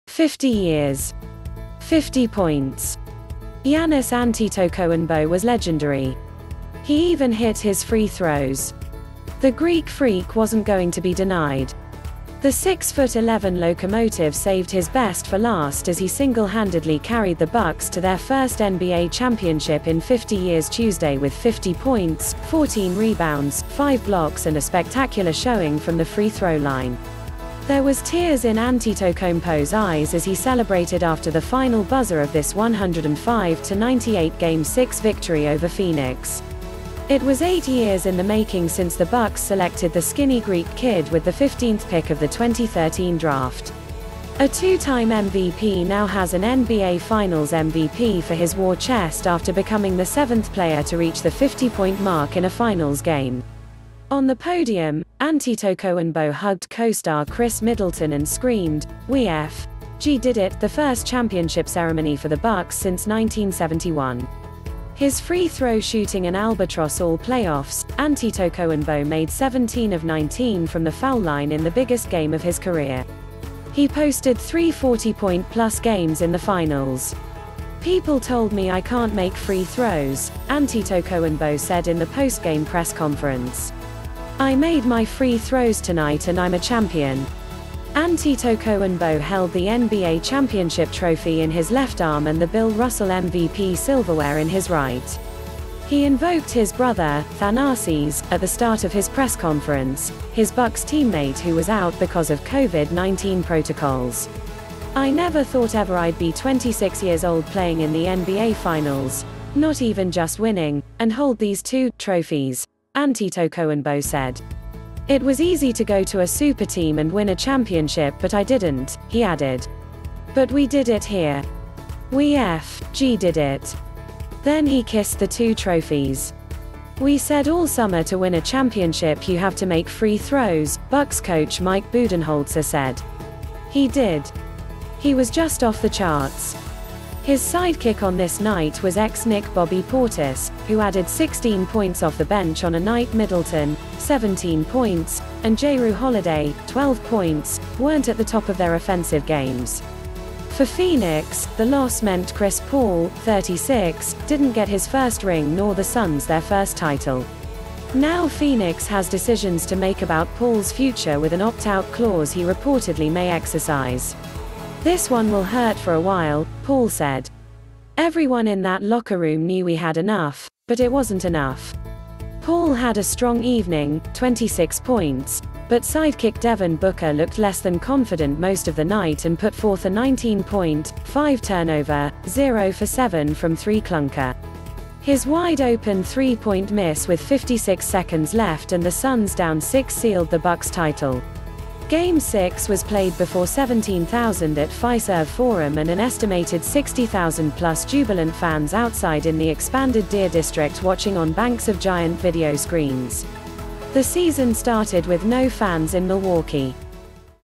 Bucks win NBA championship behind legendary Giannis Antetokounmpo
Fifty years. Fifty points. Giannis Antetokounmpo was legendary. He even hit his free throws. The Greek Freak wasn’t going to be denied. The 6-foot-11 locomotive saved his best for last as he single-handedly carried the Bucks to their first NBA championship in 50 years Tuesday with 50 points, 14 rebounds, five blocks and a spectacular showing from the free-throw line. There was tears in Antetokounmpo’s eyes as he celebrated after the final buzzer of this 105-98 Game 6 victory over Phoenix. It was eight years in the making since the Bucks selected the skinny Greek kid with the 15th pick of the 2013 draft. A two-time MVP now has an NBA Finals MVP for his war chest after becoming the seventh player to reach the 50-point mark in a Finals game. On the podium, Antetokounmpo hugged co-star Khris Middleton and screamed “We f—–g did it’’ — the first championship ceremony for the Bucks since 1971. His free-throw shooting an albatross all playoffs, Antetokounmpo made 17 of 19 from the foul line in the biggest game of his career. He posted three 40-point-plus games in the Finals.“People told me I can’t make free throws, ’’ Antetokounmpo said in the postgame press conference. “I made my free throws tonight and I’m a champion. ’’Antetokounmpo held the NBA championship trophy in his left arm and the Bill Russell MVP silverware in his right. He invoked his brother, Thanasis, at the start of his press conference — his Bucks teammate who was out because of COVID-19 protocols.“I never thought ever I’d be 26 years old playing in the NBA Finals — not even just winning — and hold these two [trophies], ’’ Antetokounmpo said.“It was easy to go to a superteam and win a championship but I didn’t, ’’ he added. “But we did it here. We f—–g did it. ’’Then he kissed the two trophies.“We said all summer to win a championship you have to make free throws, ’’ Bucks coach Mike Budenholzer said. “He did. He was just off the charts. ’’His sidekick on this night was ex-Knick Bobby Portis, who added 16 points off the bench on a night Middleton (17 points) and Jrue Holiday (12 points) weren’t at the top of their offensive games. For Phoenix, the loss meant Chris Paul, 36, didn’t get his first ring nor the Suns their first title. Now Phoenix has decisions to make about Paul’s future with an opt-out clause he reportedly may exercise.“This one will hurt for a while, ’’ Paul said. “Everyone in that locker room knew we had enough, but it wasn’t enough. ’’Paul had a strong evening (26 points), but sidekick Devin Booker looked less than confident most of the night and put forth a 19-point, five-turnover, 0-for-7-from-3 clunker. His wide-open 3-point miss with 56 seconds left and the Suns down six sealed the Bucks’ title. Game 6 was played before 17, 000 at Fiserv Forum and an estimated 60, 000-plus jubilant fans outside in the expanded Deer District watching on banks of giant video screens. The season started with no fans in Milwaukee.